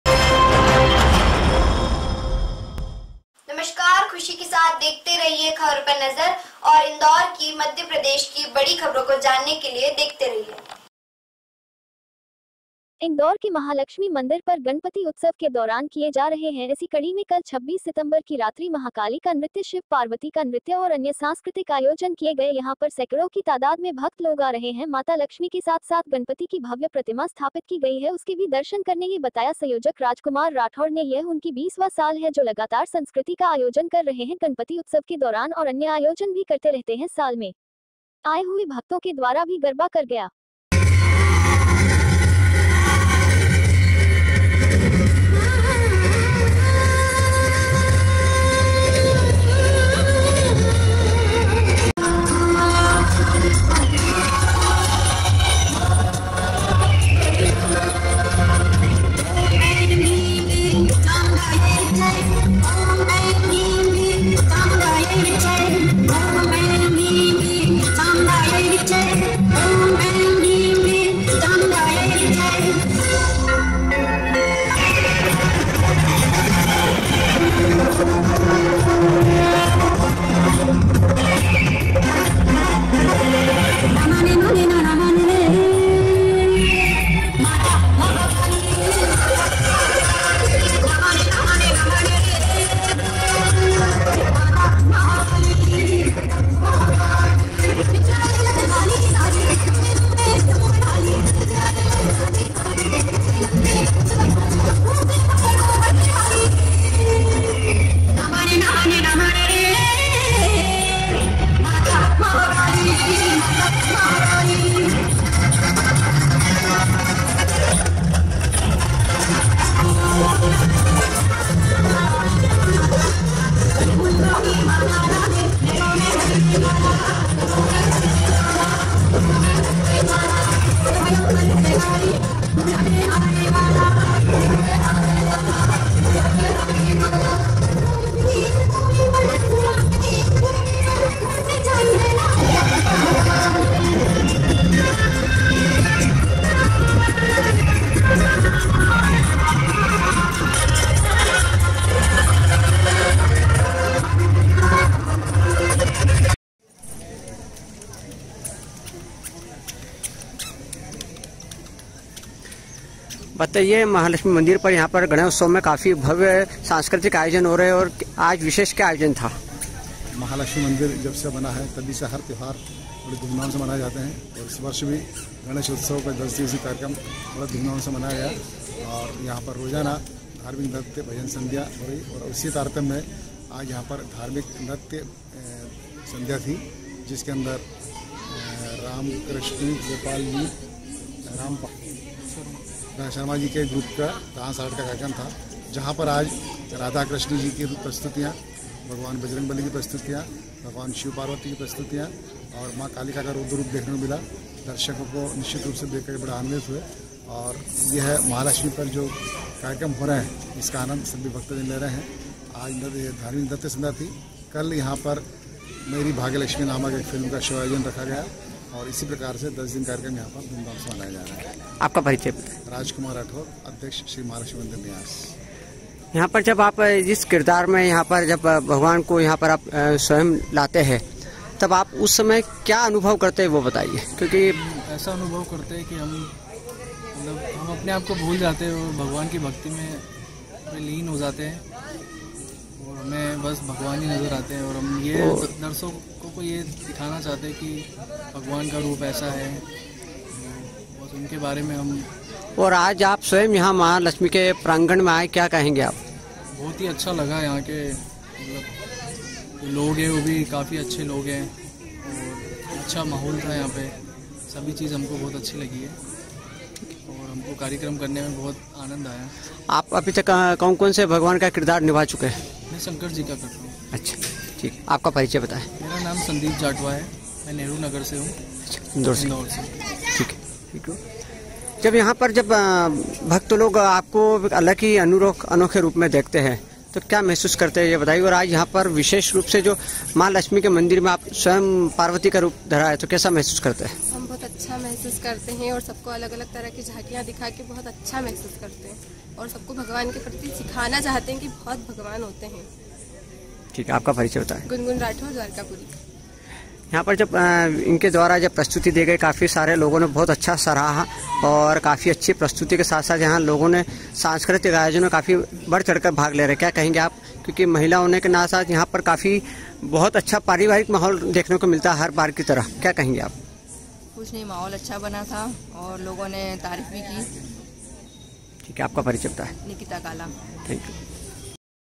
इंदौर के महालक्ष्मी मंदिर पर गणपति उत्सव के दौरान किए जा रहे सांस्कृतिक आयोजन | INDORE LAXMI MANDIR
online earn money short time